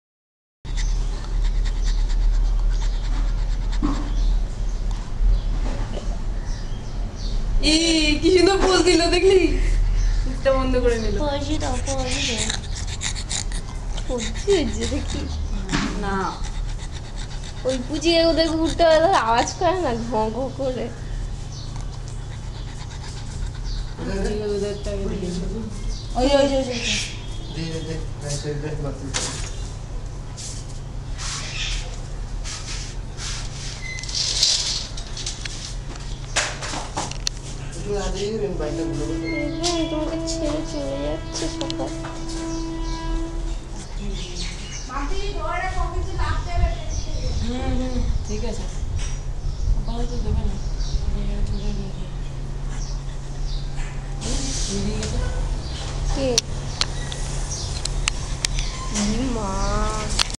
Pappy pug.
This a pug dog of our home.He is so cute,sweet puppy pug.His name is Haven.